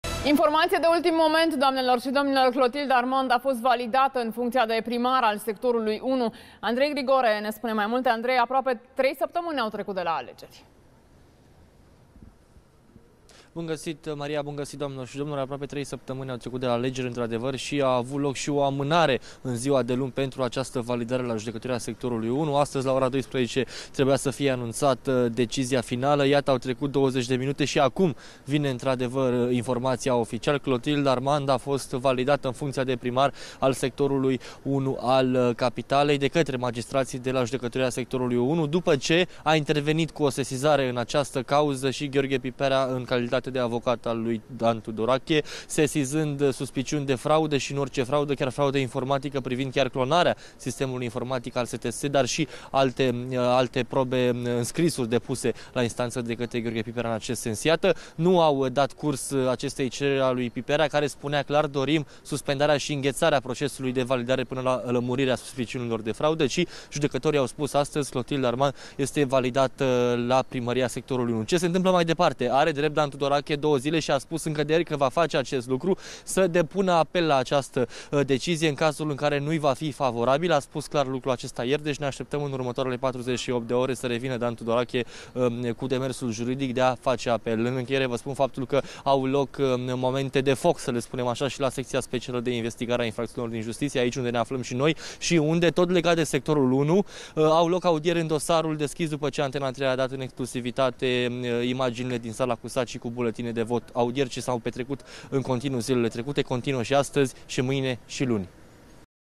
Clotilde Armand, validată primar al Sectorului 1
Judecătoria Sectorului 1 a decis, joi, în privinţa scrutinului pentru primăria de sector, grevat de numeroase suspiciuni de fraudă. Astfel, potrivit deciziei instanţei, Clotilde Armand este noul primar de sector.